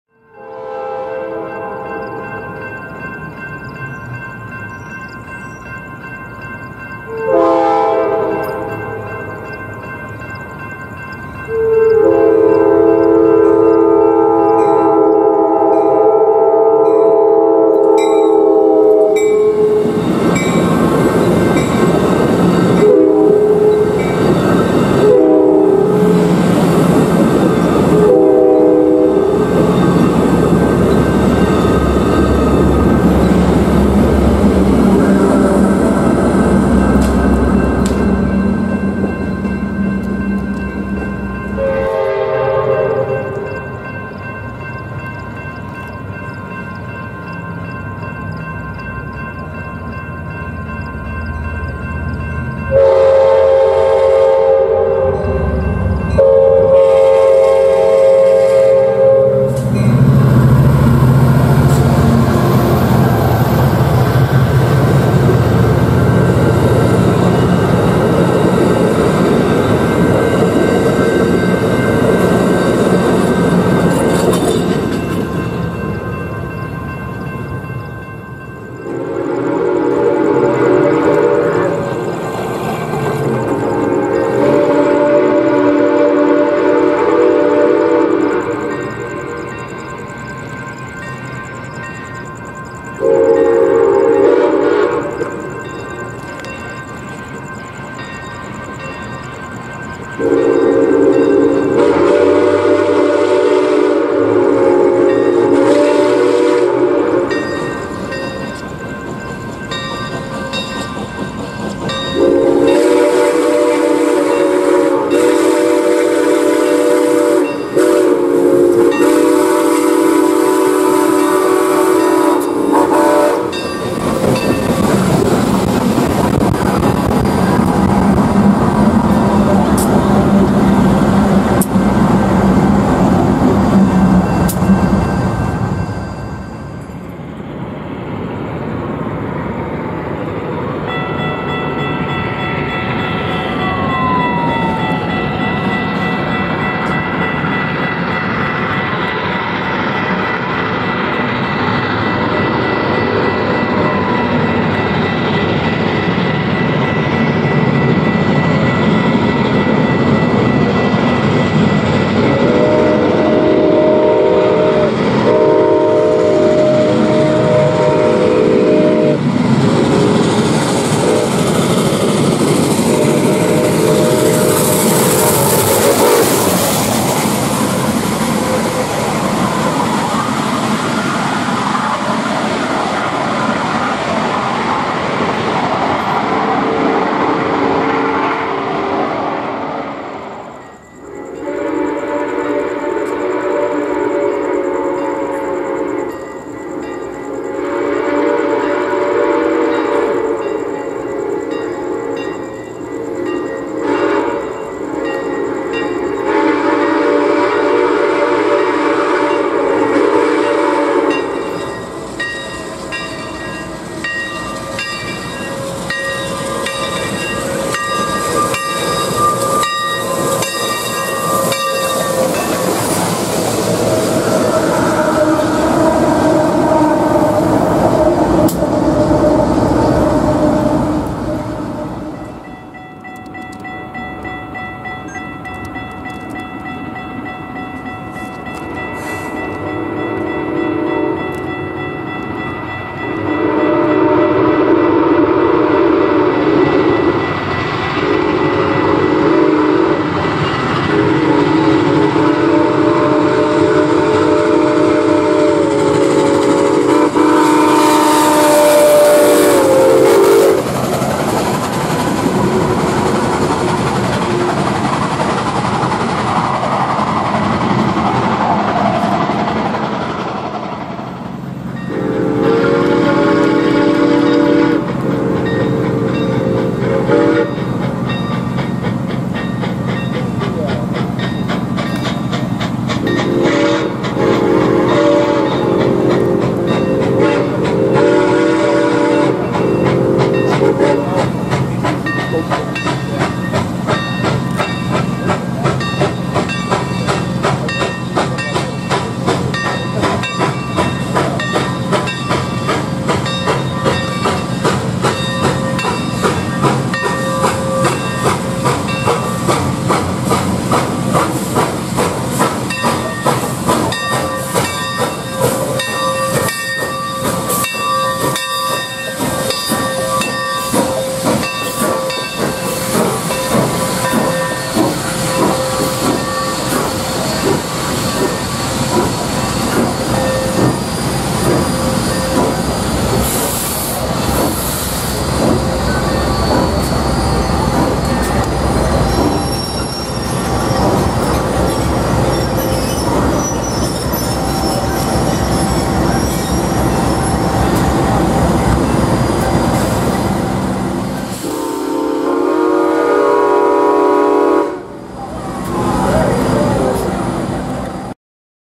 Santa Fe 3751 travels through the Antelope Valley!
History was made this week as ATSF #3751 was the first steam locomotive to travel the Antelope Valley in 32 years! Metrolink built a new platform in Acton and they asked the crew of 3751 to join them in officially opening the new platform. We begin our chase in Los Angeles, CA and we beat it (Barely) to multiple spots in the Antelope Valley. Sporting her amazing Santa Fe 6 chime whistle, the crew of ATSF #3751 did not hesitate making their presence known through the towns. I hope you enjoy the video!